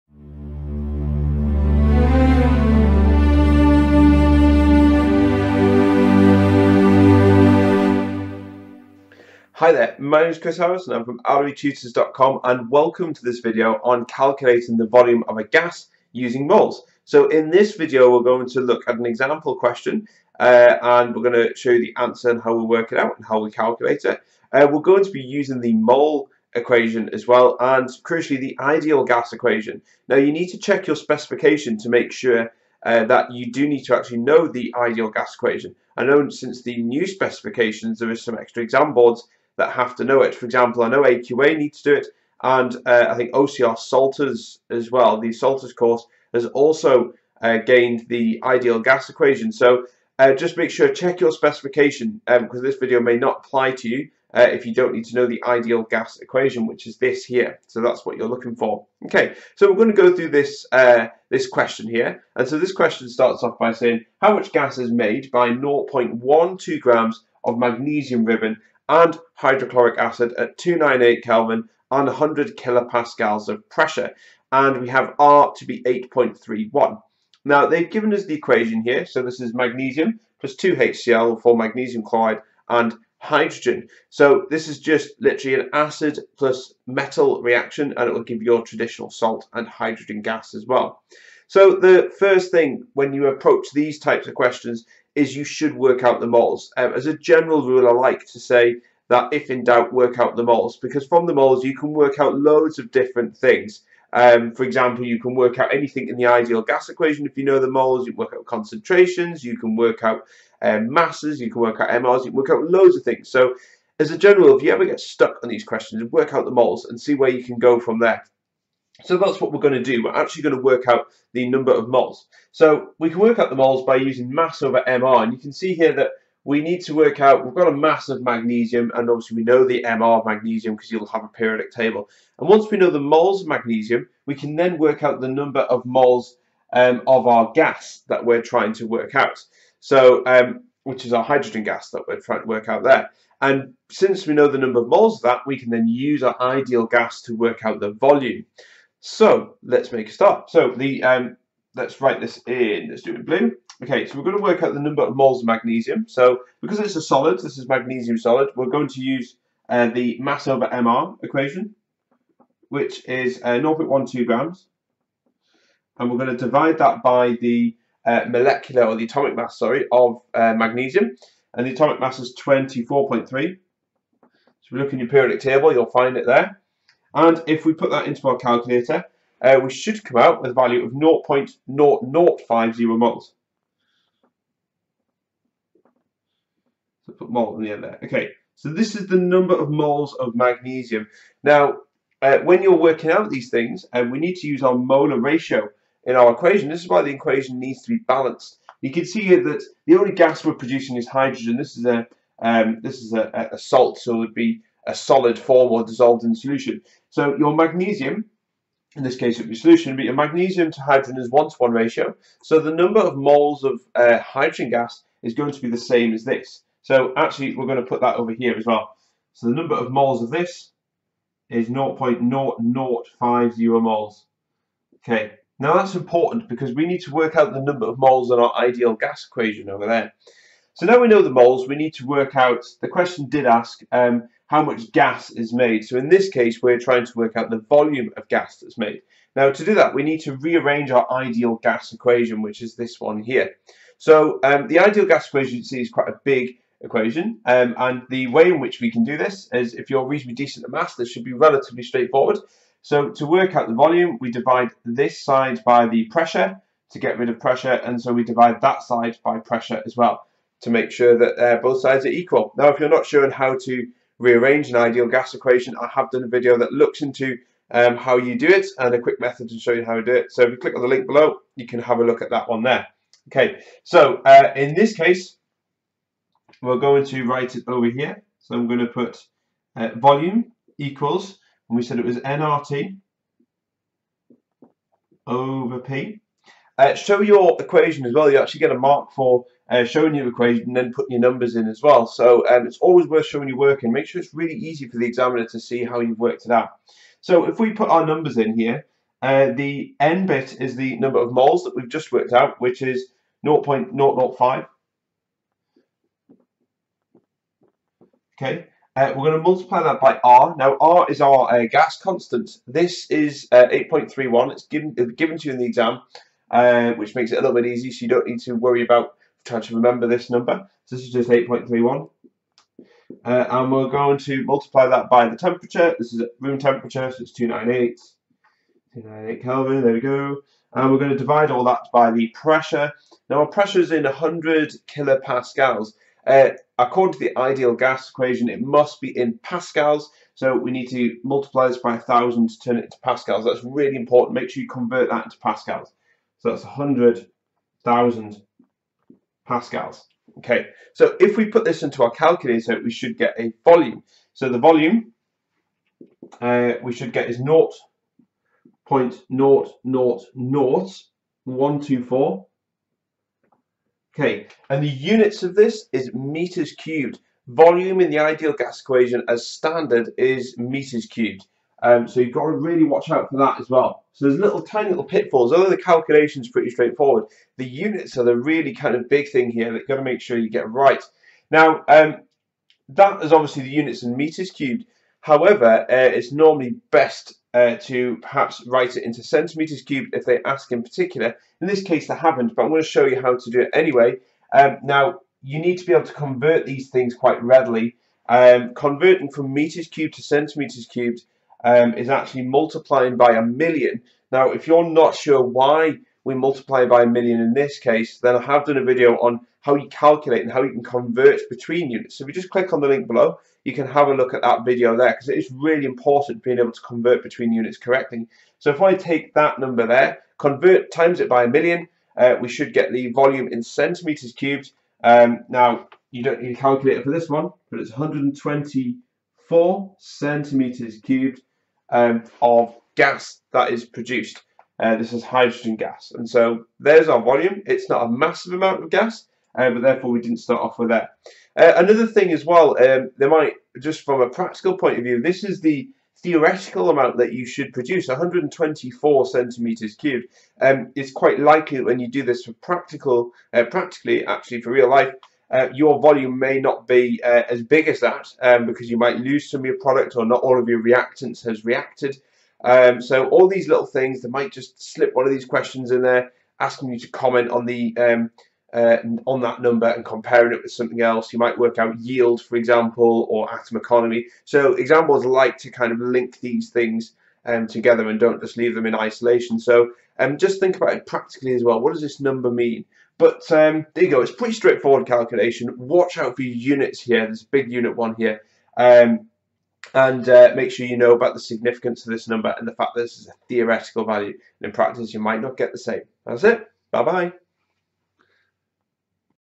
Calculating the volume of a gas using moles
A quick look at a calculation that includes 2 mole equations. A little trickier version but a one that will just add more tools in your mole calculations tool kit!